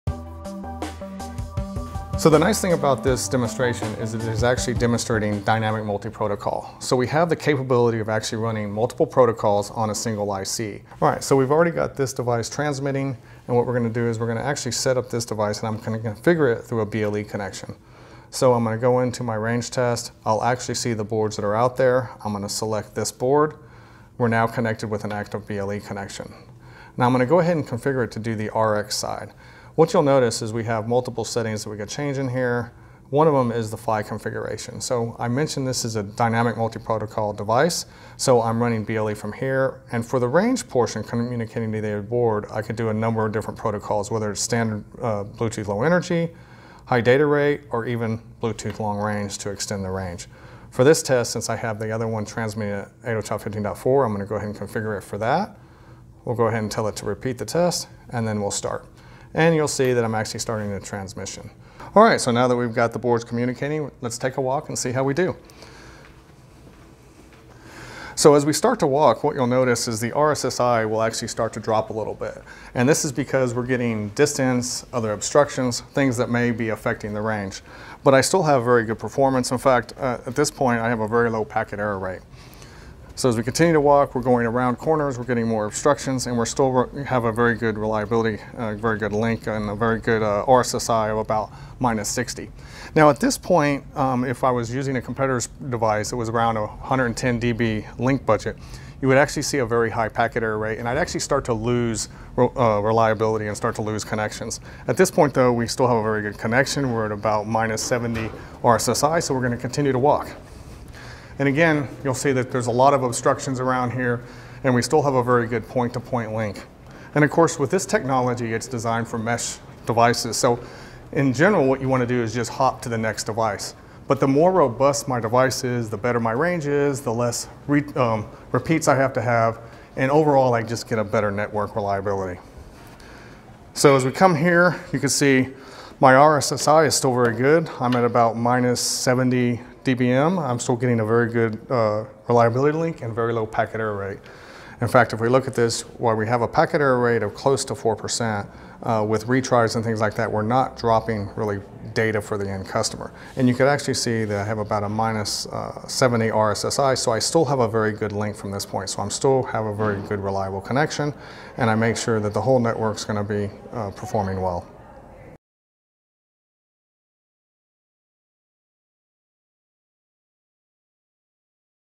Extending Zigbee, Thread, and Bluetooth Wireless Range with Series 2
Matt Maupin demonstrates the range of our new Wireless Gecko platform, Series 2.